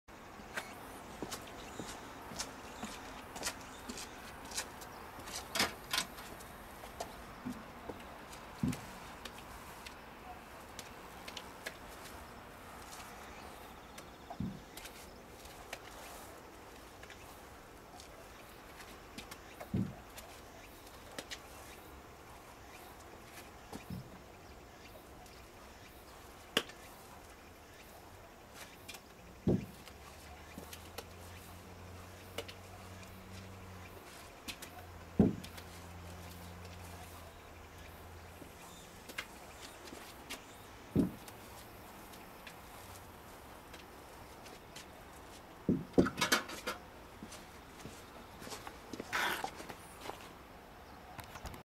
安八郡 Ｋ様 防水工事 岐阜県＠中村ワークス
防水工事で中塗り（防水材）という工程をしております。